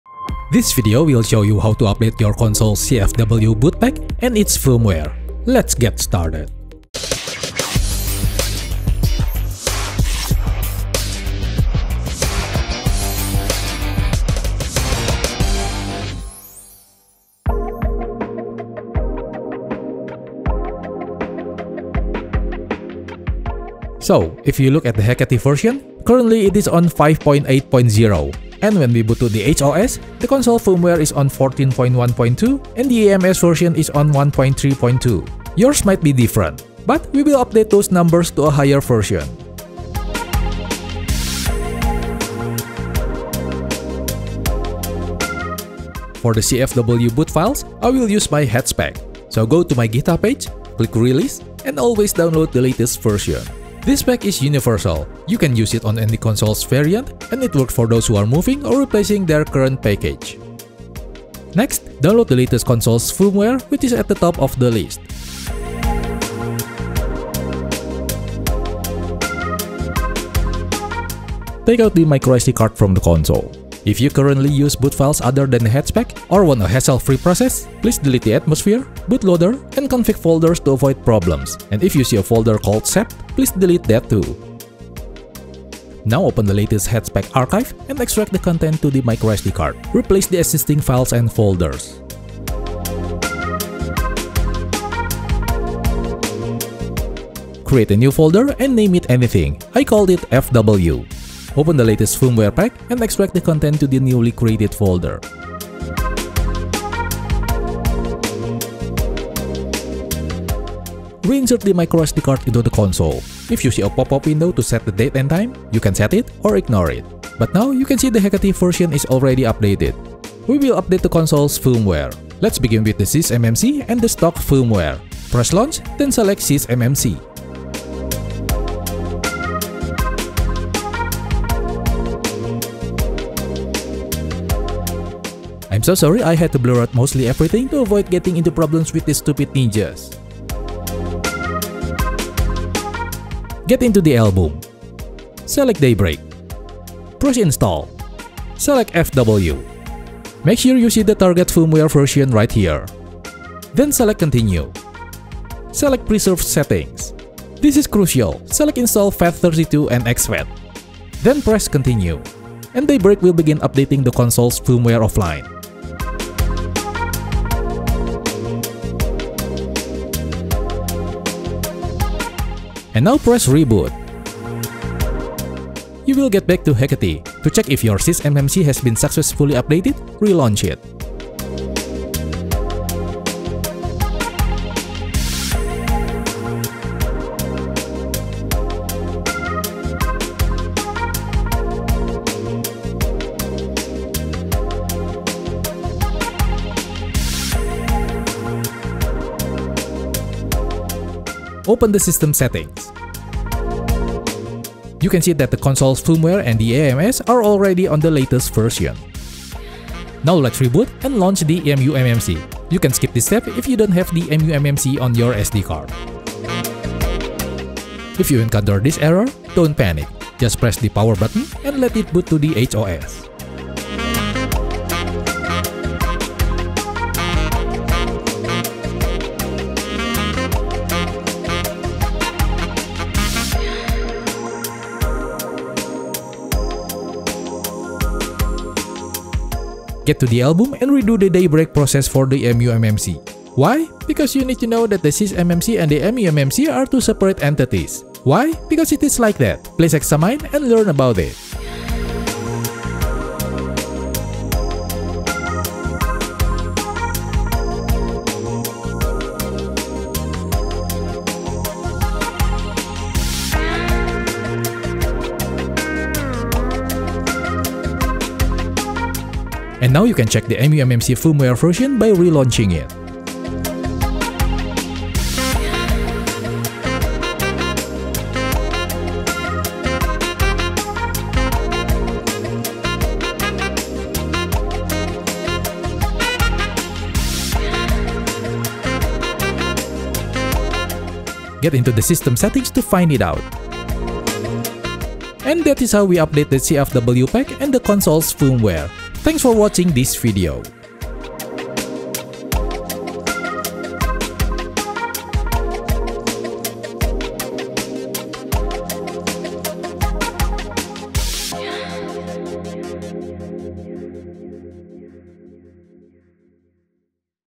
UPDATING THE HATS + AMS + FW SAFELY
This video will show you how to safely update the HATS pack, the AMS, and the FW. It works on any console's variants, and I can say it is future-proof.
MUSIC

LINKS